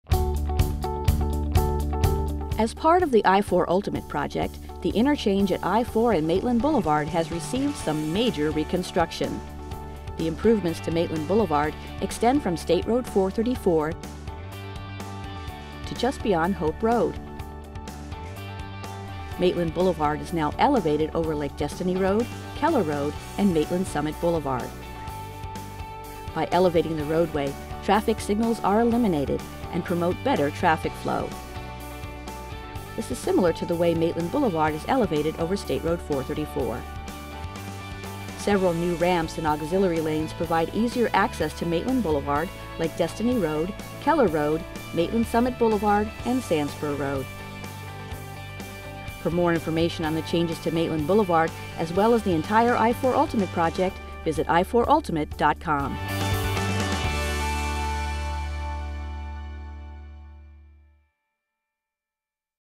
I-4 Ultimate: MAITLAND BLVD. IMPROVEMENTS
Take a look at some of the improvements – including roadway elevations and new lanes – designed to ease congestion and increase traffic flow along Maitland Boulevard (State Road 414) at its interchange with Interstate 4. Elevating the road over intersections eliminates several traffic signals, increasing safety and mobility in this heavily traveled corridor.